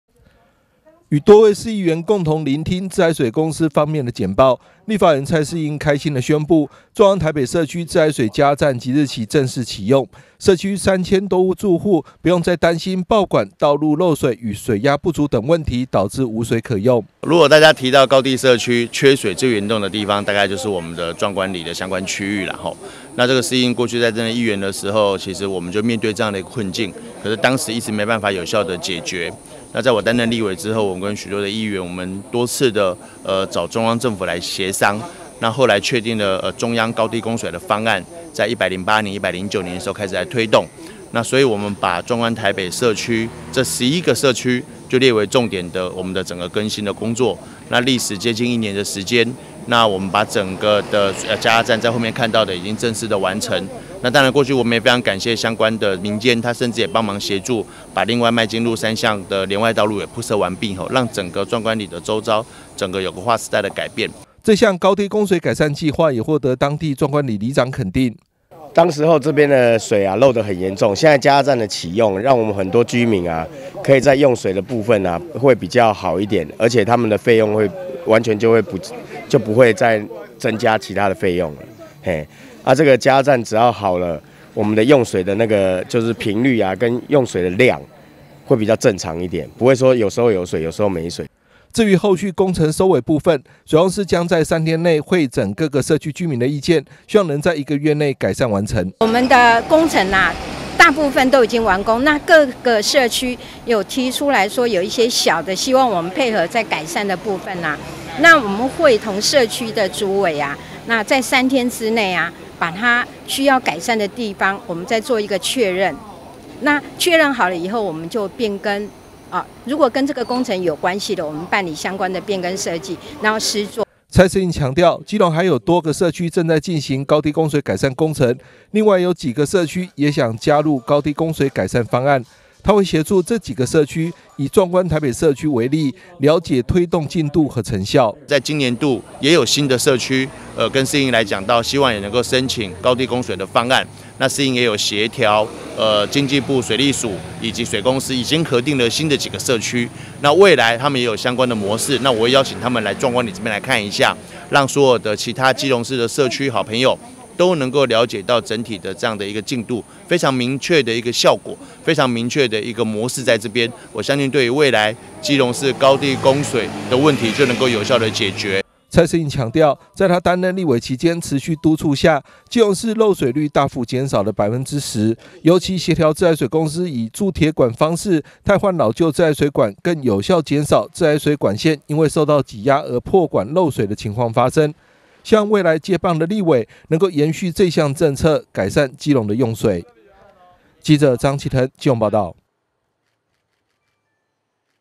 自來水 加壓站|蔡適應 基隆|爆管 漏水率|高地 供水|汰舊 換管|2023 中嘉新聞|水公司全面接管 自來水加壓站啟用 壯觀里三千住戶用水無虞
壯觀台北社區三千戶受惠 立委蔡適應完成議員蔡適應政見 自來水加壓站啟用

在立法委員蔡適應積極爭取推動老舊社區高地供水改善計畫下，壯觀台北社區自來水加壓站5/29正式啟用，社區內三千多戶所使用的自來水管線都由自來水公司接管維養，未來因為爆管、道路漏水、水壓不足等問題導致社區無水可用的情況將不再發生。

與多位市議員共同聆聽自來水公司方面的簡報，立法委員蔡適應開心地宣布壯觀台北社區自來水加壓站即日起正式啟用，社區三千多戶住戶不用再擔心爆管、道路漏水與水壓不足等問題導致無水可用。

這項高地供水改善計畫也獲得當地壯觀里里長肯定。

至於後續工程收尾部分，水公司將在三天內彙整各個社區居民的意見，希望能在一個月內改善完成。

蔡適應強調基隆還有多個社區正在進行高地供水改善工程，另外有幾個社區也想加入高地供水改善方案，他會協助這幾個社區，以壯觀台北社區為例，了解推動進度和成效。

蔡適應強調在他擔任立委期間持續督促下，基隆市漏水率大幅減少了10％，尤其協調自來水公司以鑄鐵管方式汰換老舊自來水管線更有效減少自來水管線因為受到擠壓而破管漏水的情況發生，希望未來接棒的立委能延續這項政策，改善基隆的用水。